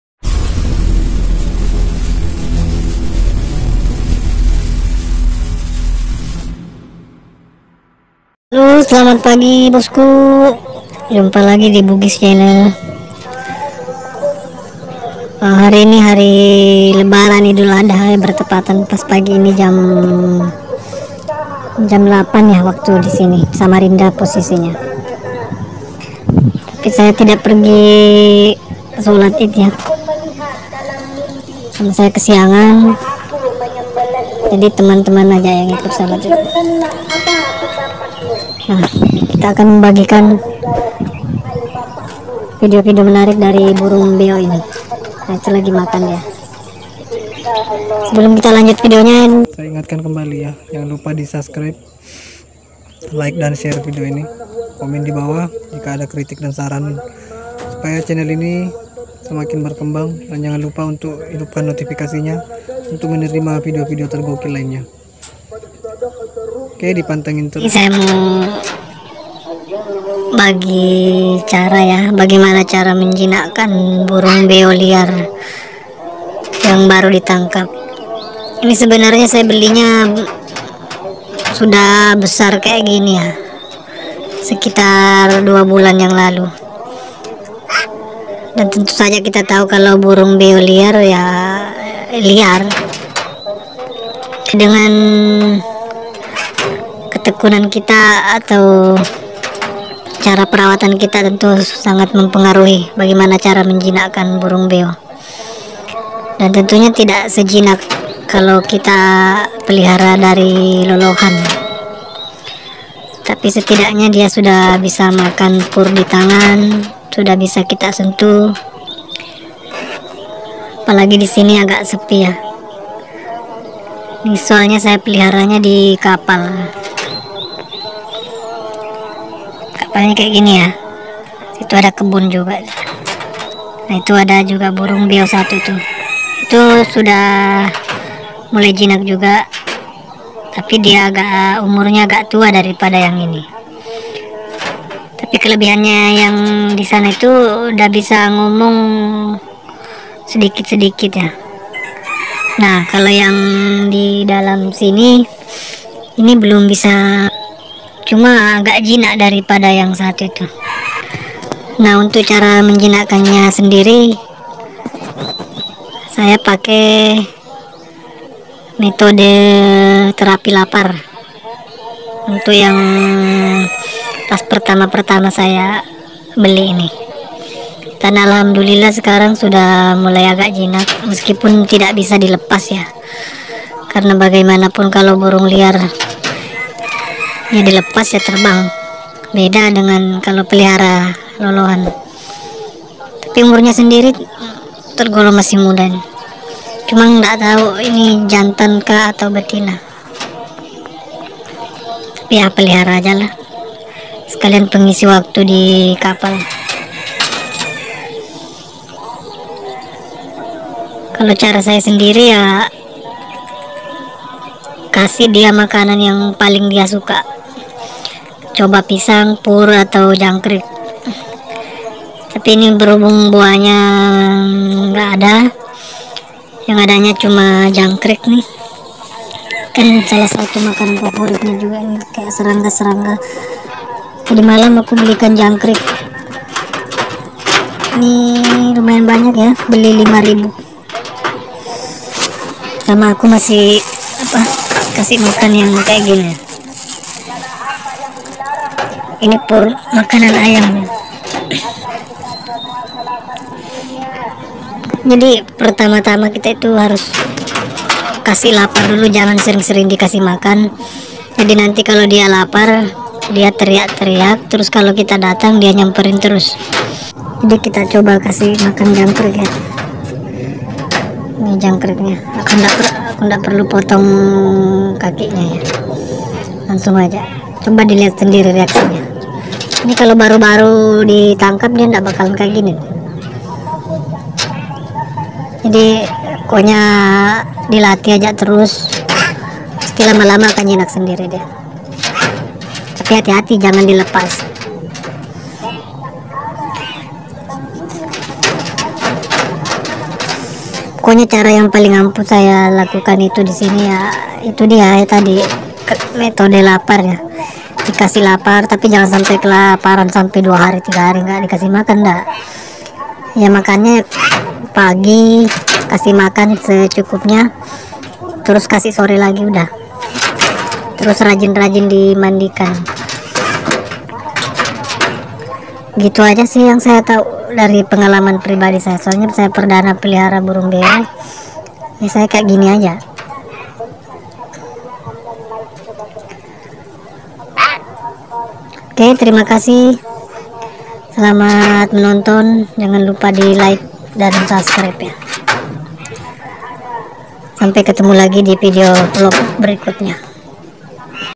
Cara cepat menjinakan burung beo dewasa // burung beo liar bisa jinak
Cara sederhana menjinakan burung beo dewasa..

Burung Beo Kalimantan memang terkenal bisa menirukan suara manusia dan suara apa aja yg sering dia dengarkan ..
burung ini bisa kita jumpai di tanah hulu pedalaman Kalimantan .
cara supaya beo bisa ngomong memang butuh waktu apalagi untuk burung beo yang sudah tua atau dewasa beda halnya dengan membeli dari lolohan ..
untuk cara saya sendiri
saya melatih burung beo untuk jinak dulu biar bisa lebih mudah pada saat di latih bicara nantinya ..

silahkan di simak video selengkapnya !

ingat !
burung beo jangan sampai stres dan jangan terlalu sering di pegang ..

cintailah hewan peliharaan seperti anda mencintai dirimu sendiri,.